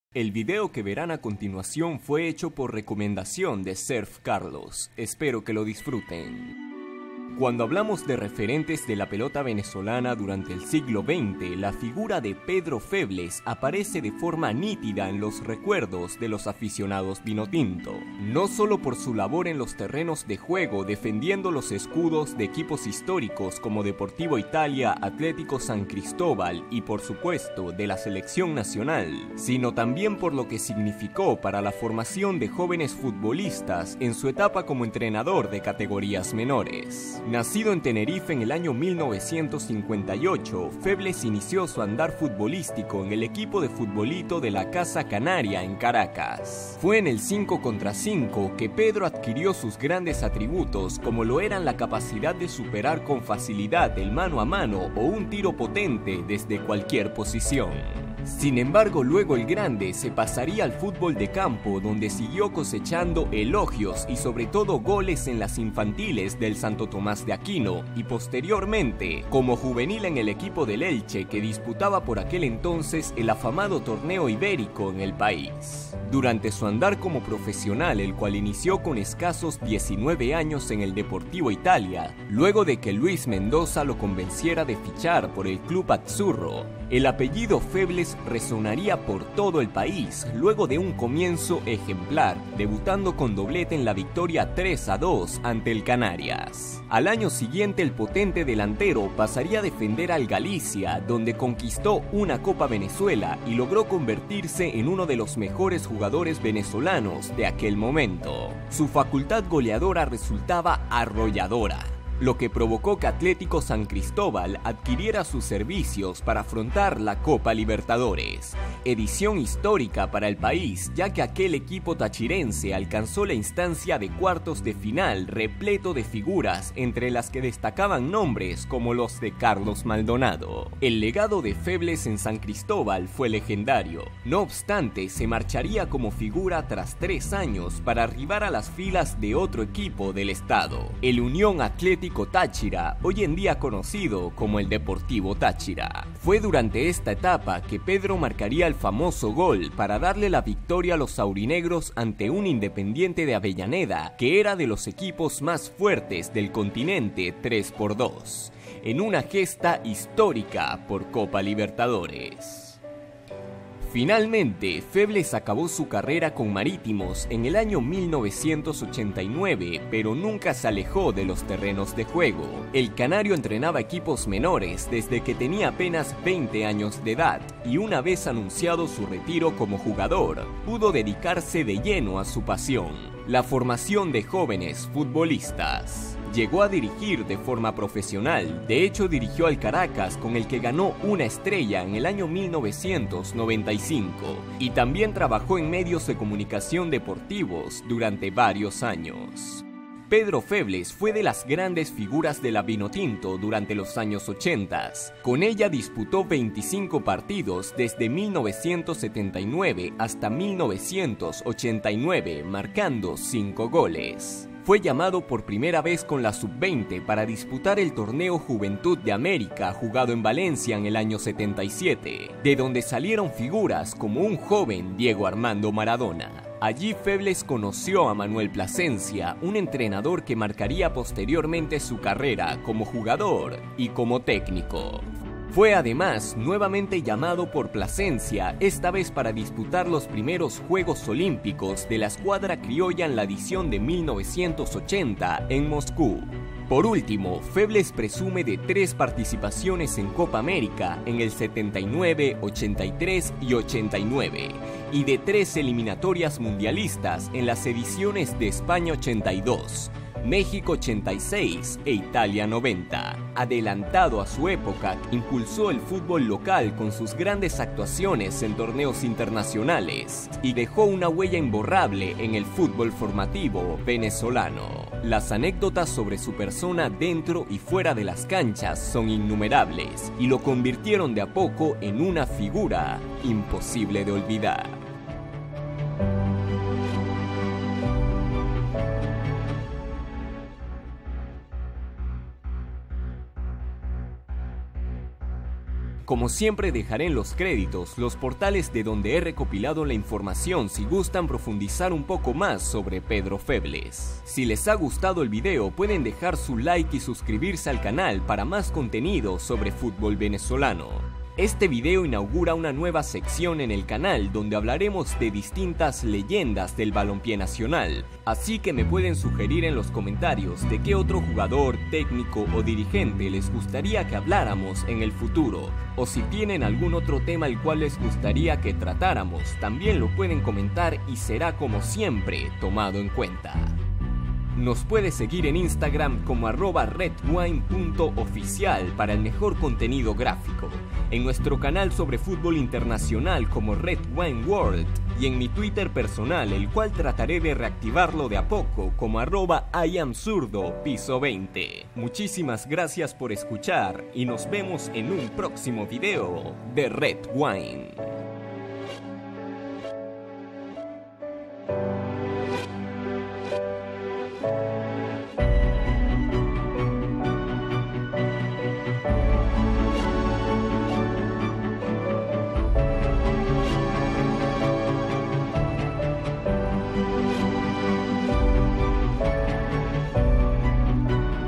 Leyendas Vinotinto: Pedro Febles | RedWine
El día de hoy conmemoramos la carrera del grande Pedro Febles. Ilustre delantero venezolano que maravilló a todo el continente en los años 80s.

¡Cuéntame qué te pareció el vídeo!

Se viene más contenido que nunca al canal
¿Que te gustaría ver en próximos videos?